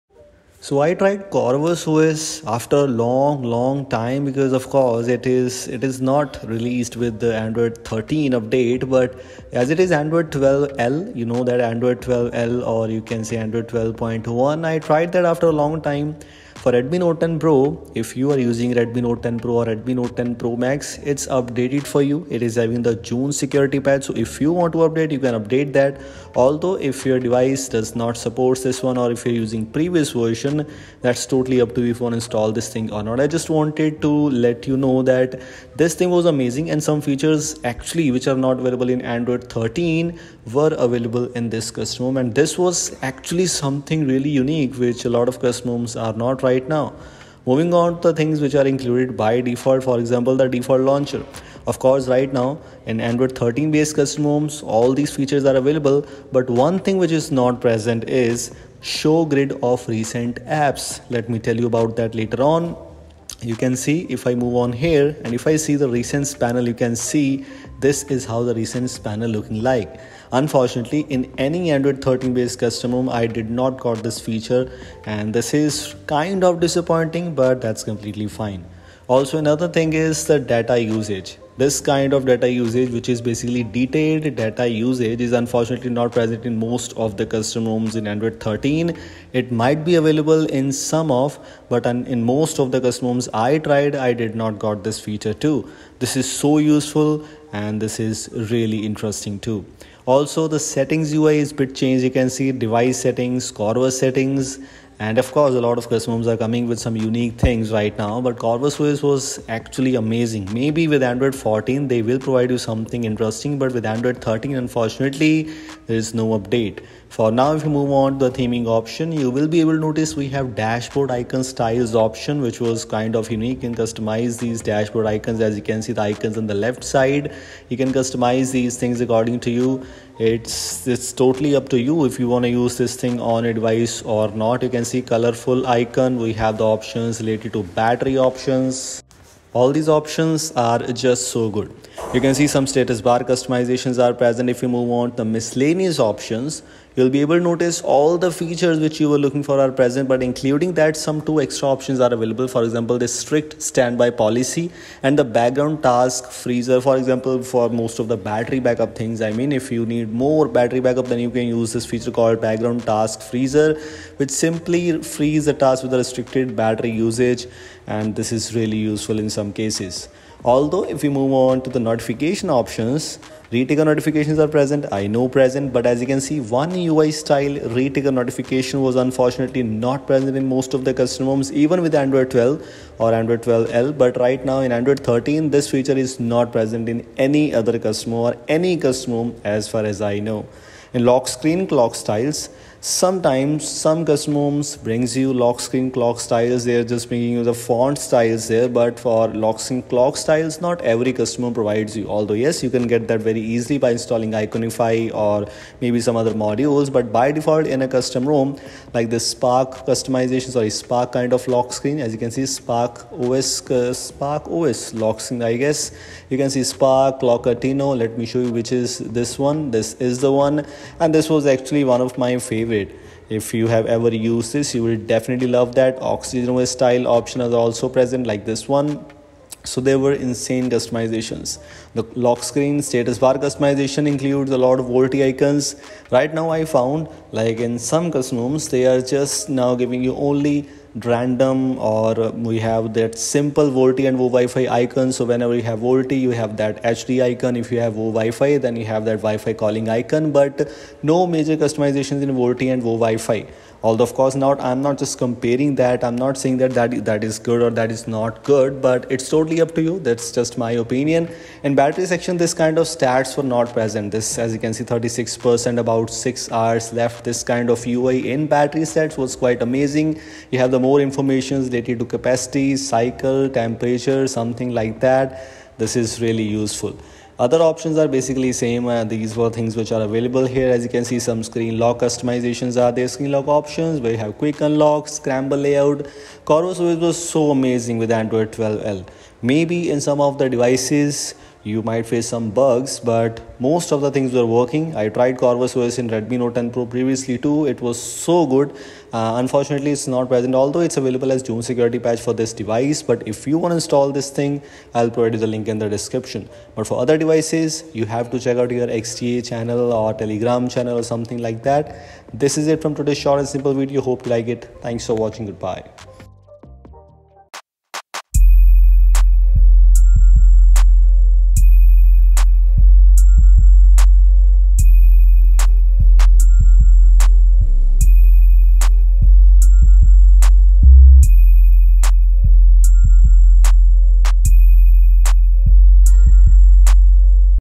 I tried Corvus OS Android 12L - It's Kind of Unique!
Hey guys, What's Up? Everything is good I Hope. In this video, I'm gonna show you " I tried Corvus OS Android 12L - It's Kind of Unique! ". Make Sure to watch this video till the end to understand everything.

Credits: Ajit